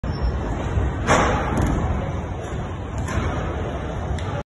INSANE BACKFLIP ON SCOOTER!
Hey, my name is Marie. I love riding pro scooters and doing flips on trampolines.

Im sorry for the bad resolution :(